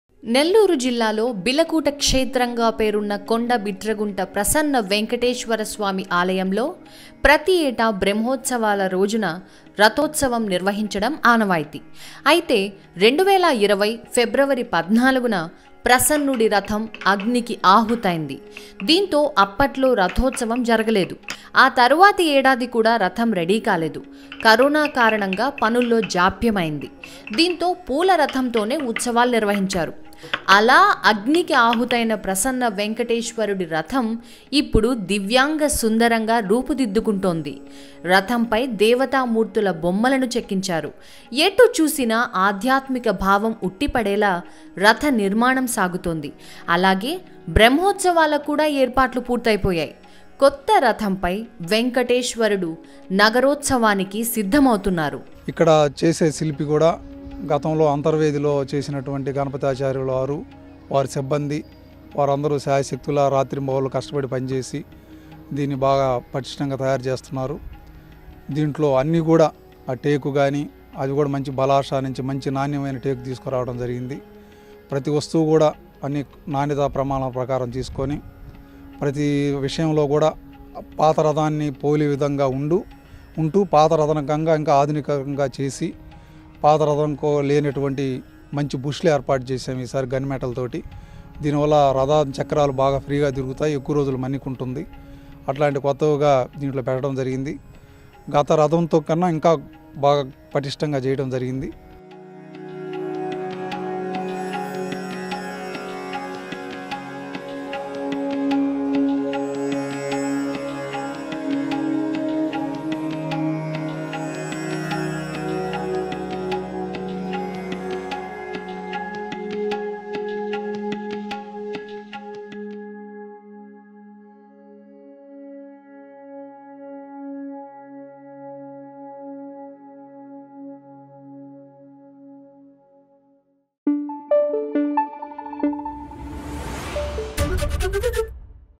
Nellore wooden chariot : wooden chariot preparations Nellore| ABP Desam
Nellore లో  కొండ బిట్రగుంట ప్రసన్న Lord Venkateswara Templeలో ప్రతి ఏటా బ్రహ్మోత్సవాల రోజున రథోత్సవం నిర్వహించడం ఆనవాయితీ. అయితే 2020 February 14న ప్రసన్నుడి chariot అగ్నికి ఆహుతైంది.